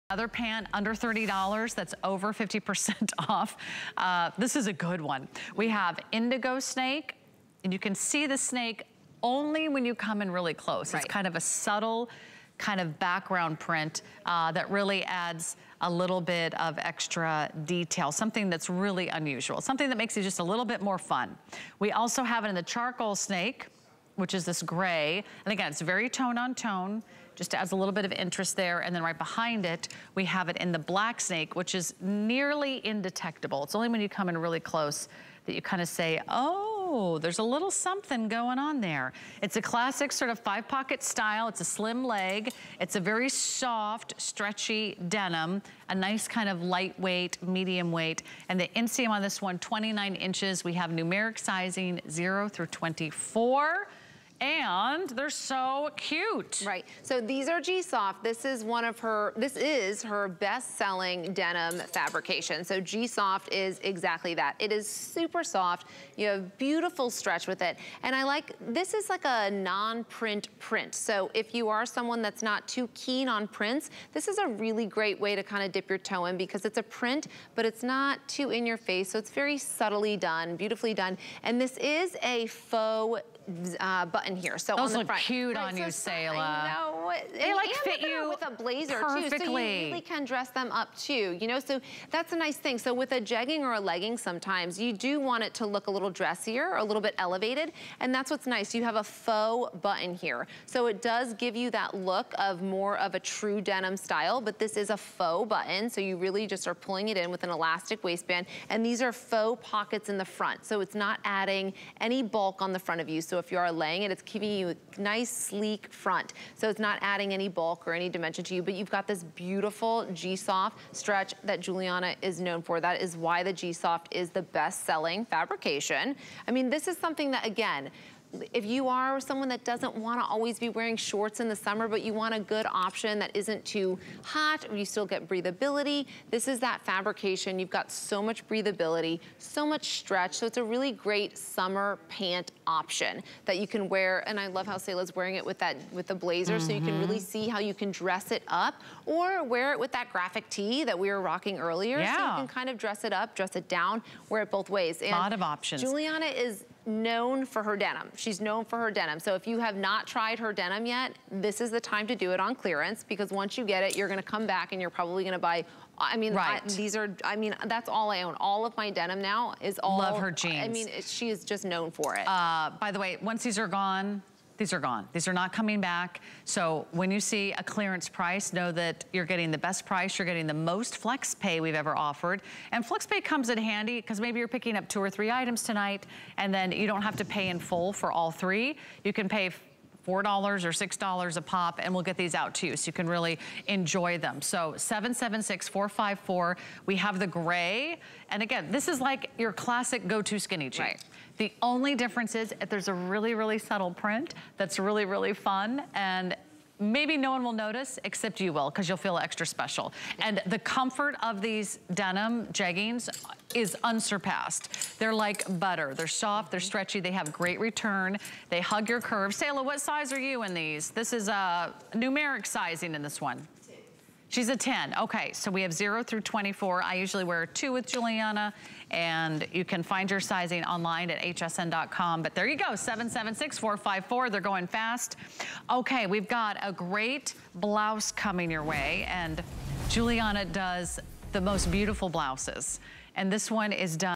G by Giuliana GSoft Faux Fly Denim Jegging
Looks like a jean, feels like a legging. A realistic faux fly and belt loops add to the classic style, while an elastic back waistband lets you ease into this fitted pullon.